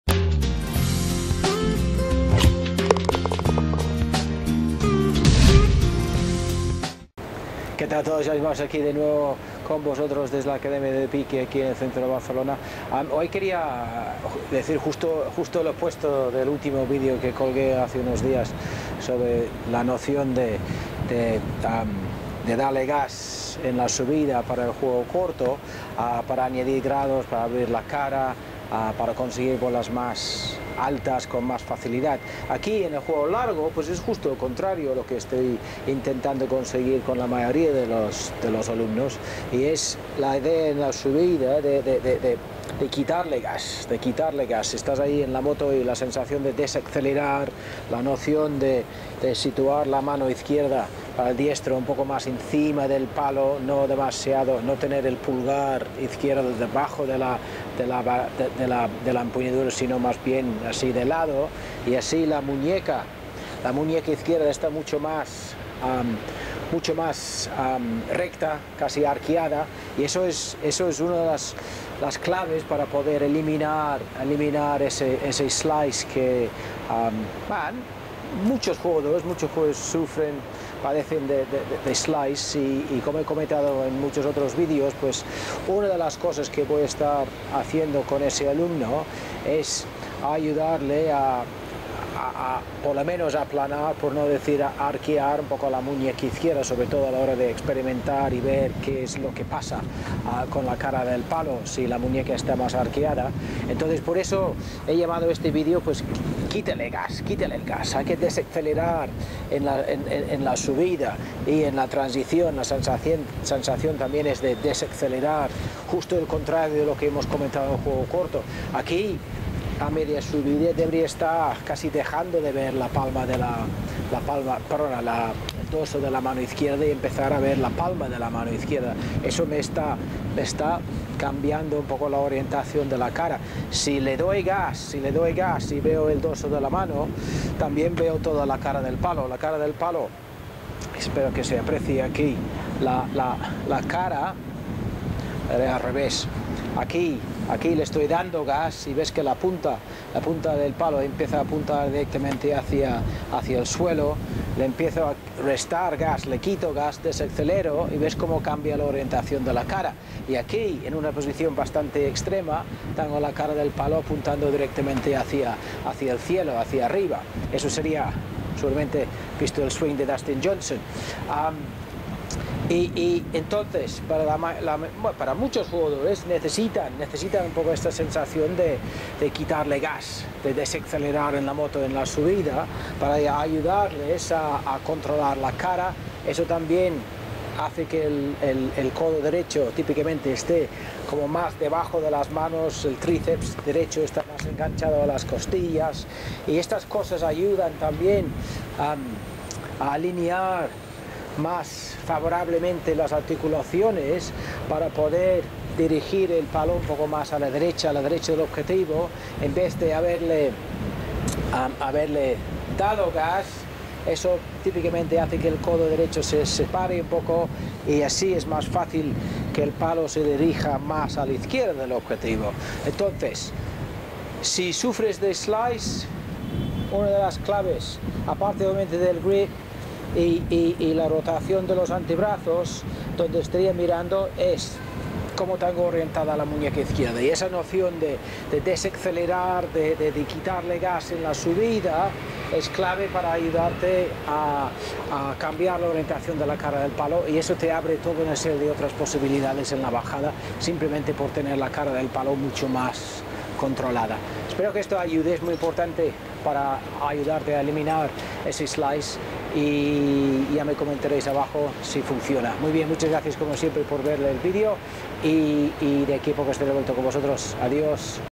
¡Quítale gas en la subida para eliminar ese slice!
Una de las claves de eliminar ese slice que tanto estropea tu habilidad de mejorar tu rendimiento en el campo es poder arquear la muñeca izquierda en la subida, quitarle gas en la moto. En este vídeo os enseño cómo aplicar esto a tu juego para empezar a eliminar tu slice enseguida y para siempre!